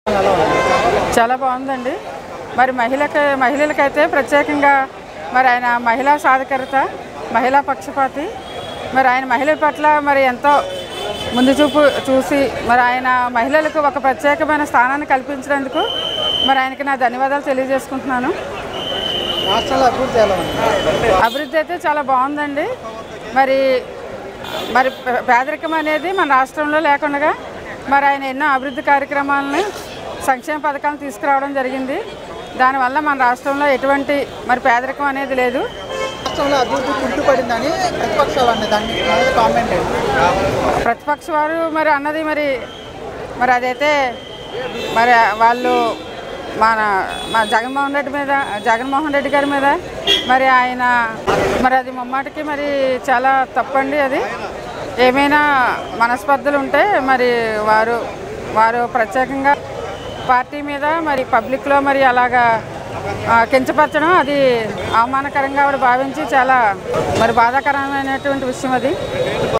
AP Public Talk : YS Jagan Development AP | Supports YSRCP Govt | Chandrababu Drama's TDP | Elections
ap news
ys jagan
ys jagan mohan reddy
ysrcp
janasena
pawan kalyan
janasena live
ysrcp live
tdp live